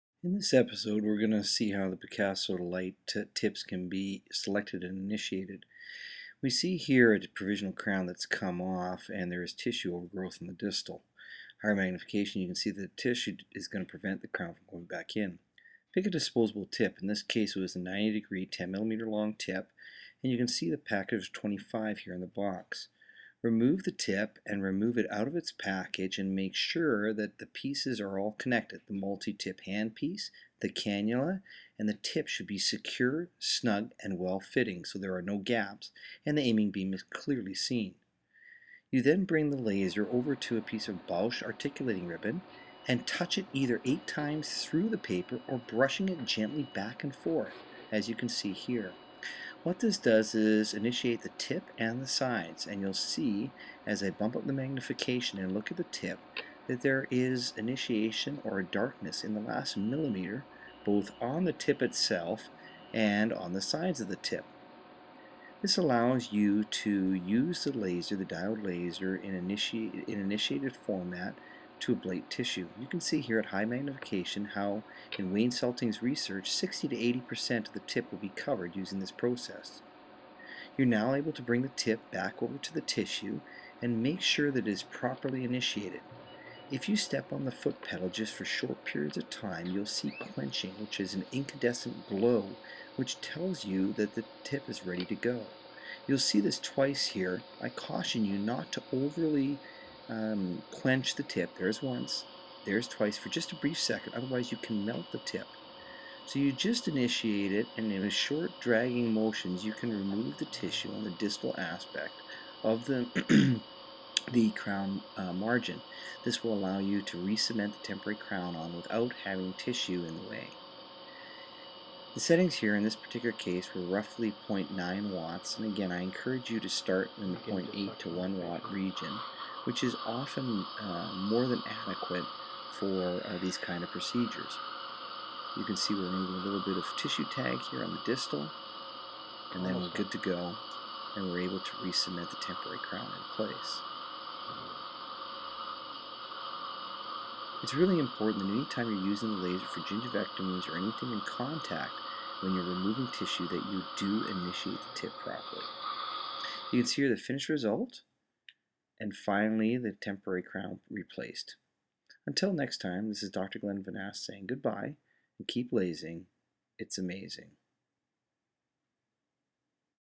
Initiating Tip.mov
In this video we see how to properly initiate the tip for the Picasso Lite lasers  on Bausch articulating ribbon to be used in contact ablation laser dental surgery.